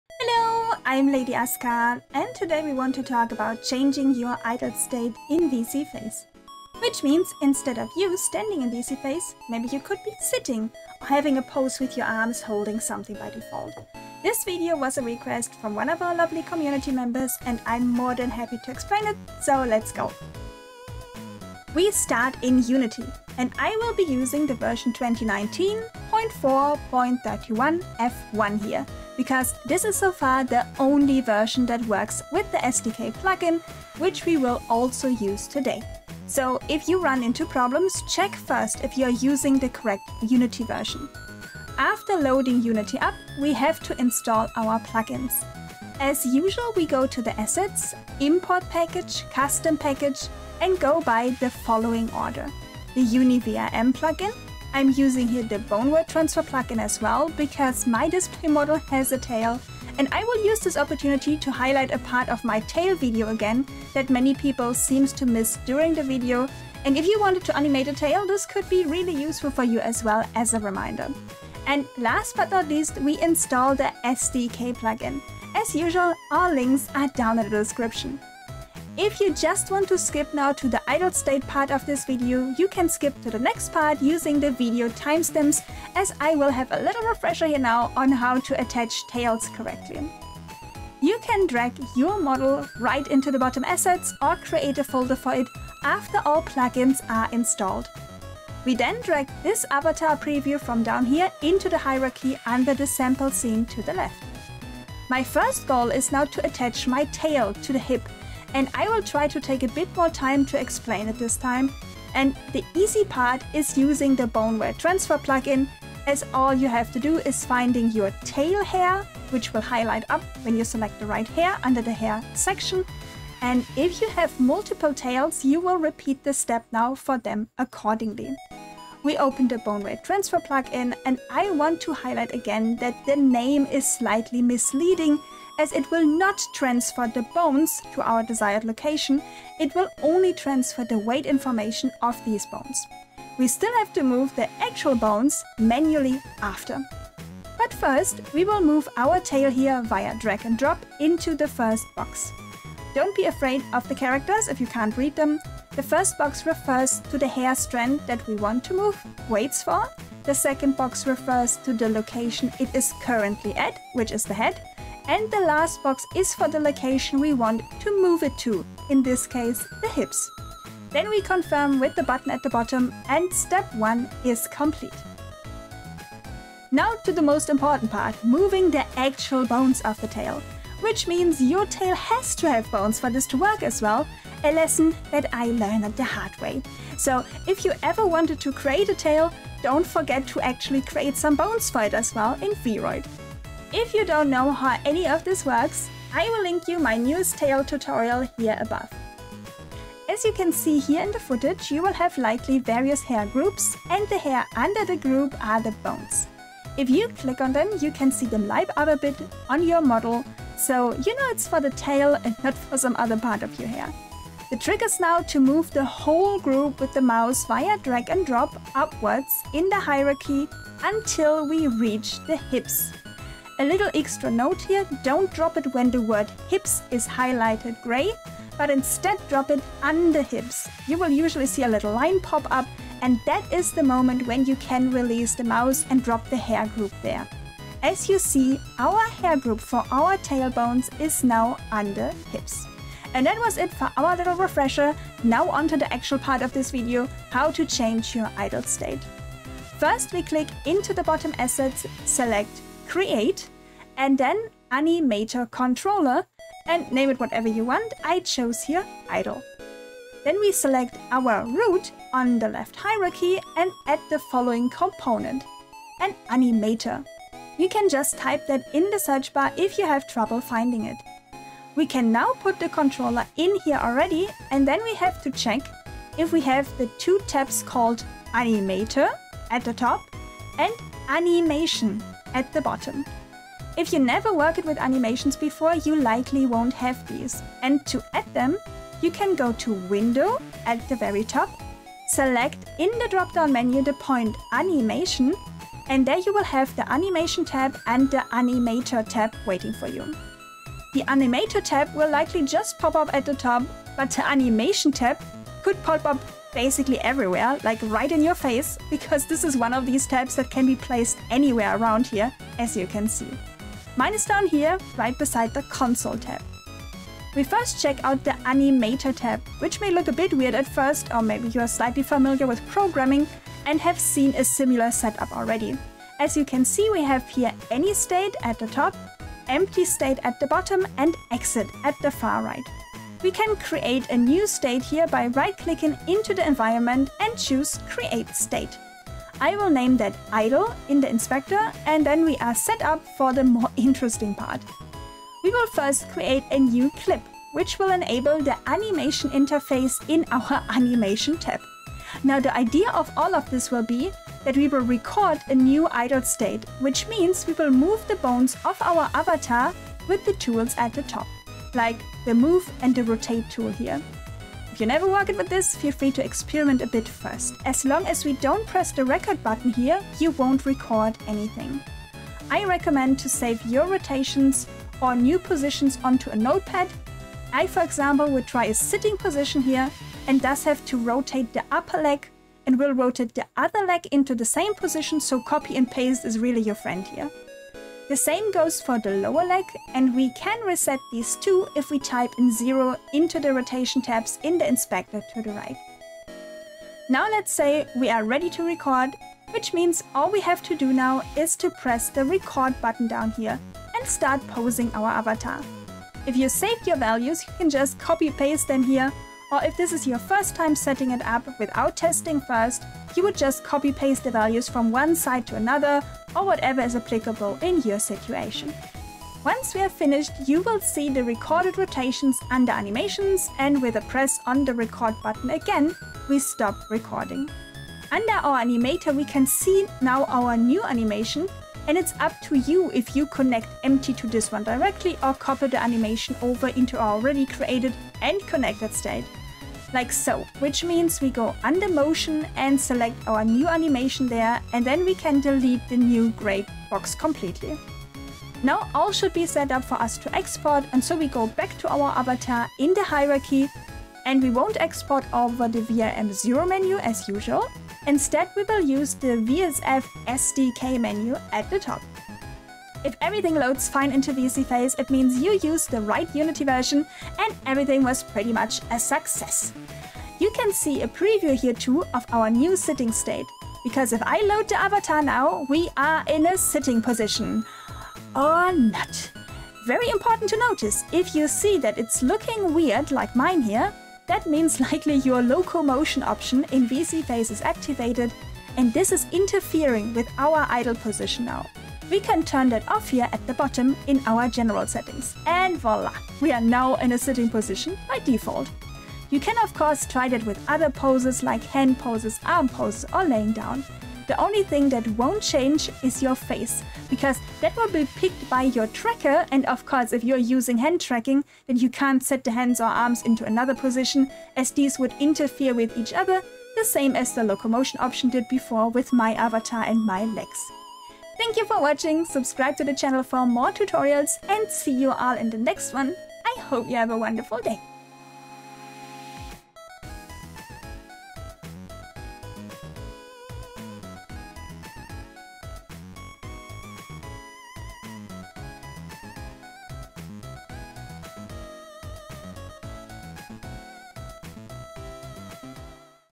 Vroid Studio Unity - How to change your Idle pose for VSeeFace
Hello! :D
We are back with a requested tutorial, about how to change your idle pose of your Vroid avatar in VSeeFace, using Unity and the SDK plugin!

🕘Timestamps:
0:00 Video Start
0:21 Preparing Unity
1:25 Attaching a Tail - refresher
4:06 Changing the idle state in Unity
7:42 Testing in VSeeFace